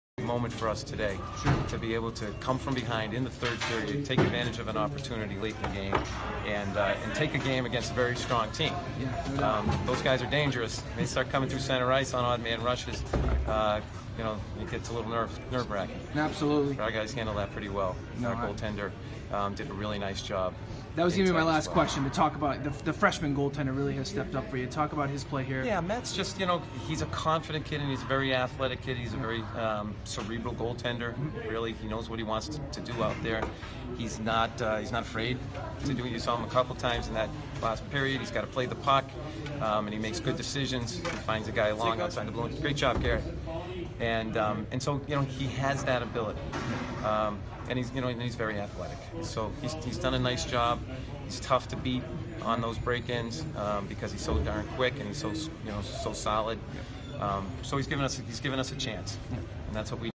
Algonquin coach Greg Cappello said being able to come from behind was an important stepping stone fo
Algonquin coach Greg Cappello said being able to come from behind was an important stepping stone for his team.

By: Chris Kyne
Published on: February 18, 2017